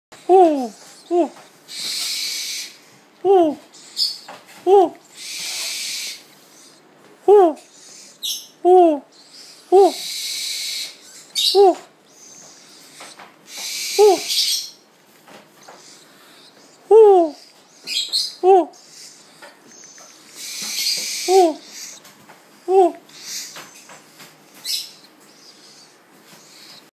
呼べばこっちを振り向くフクロウのヒナ達
スピックスコノハズクのヒナ達です。目が見えないので声が聞こえる方を向く訳です。

フクロウを本気で飼いたい方やご興味のある方は下記アドレスへどうぞ
By 神戸ふくろう館　猛禽倶楽部